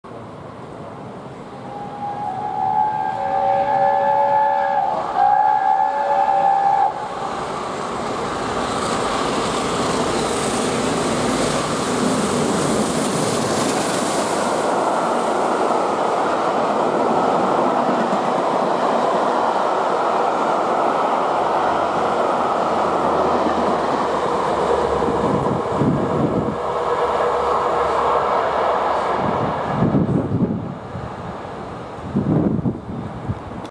60163 Tornado, The Cumbrian Mountaineer Tour, Sat. 16th March 2019
Seen at Lancaster, after being diverted from the original route back south over the Settle and Carlisle line.